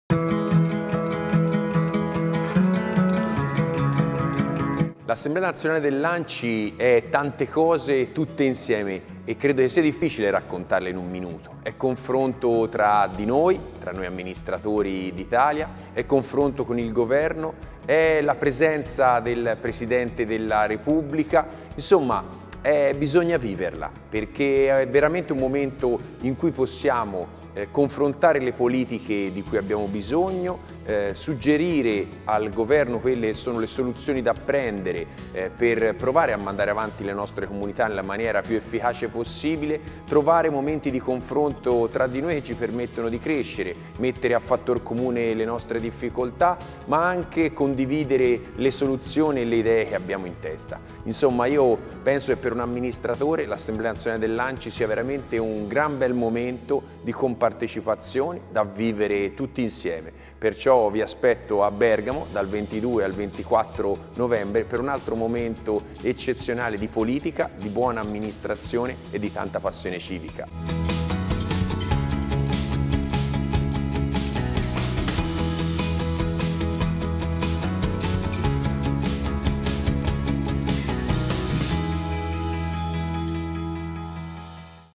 L'appello di Biffoni: "L'assemblea di Anci un momento di buona politica per condividere esperienze"
"Crescere e metter a fattor comune le nostre difficoltà ma anche le soluzioni per le nostre comunità. Quello dell'Assemblea sarà un momento di compartecipazione da vivere tutti assieme. Un momento di buona amministrazione e passione civica". Matteo Biffoni invita con queste parole i colleghi sindaci a partecipare ai lavori della XXXIX assemblea annuale dell'Anci che si aprirà il 22 novembre a Bergamo per chiudersi giovedì 24.
"Un momento quello dell'assemblea - aggiunge Biffoni - dove confrontare le diverse politiche dei territori che ogni giorno gli amministratori si trovano a mettere in campo".